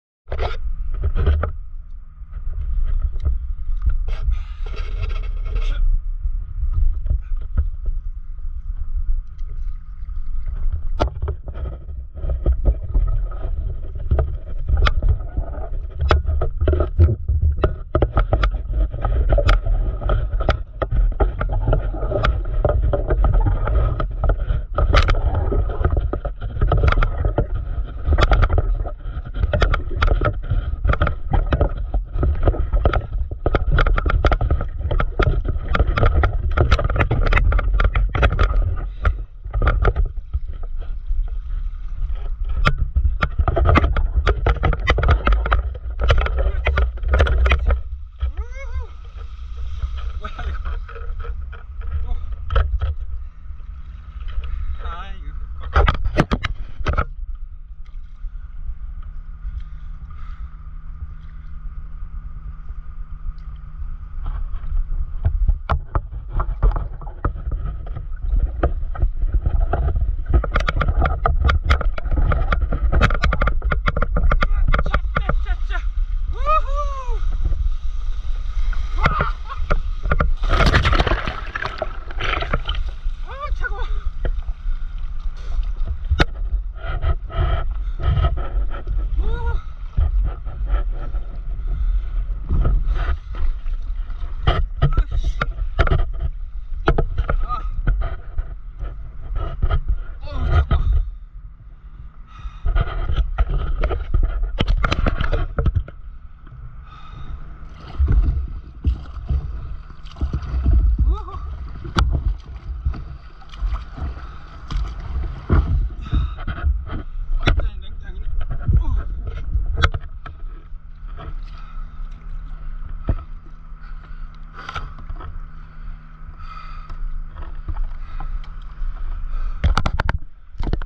만리포 서핑, 영상 찍기 어렵네 !
패들보드 레이싱 훈련 갔다가 파도 들어와 서핑 모드로 전환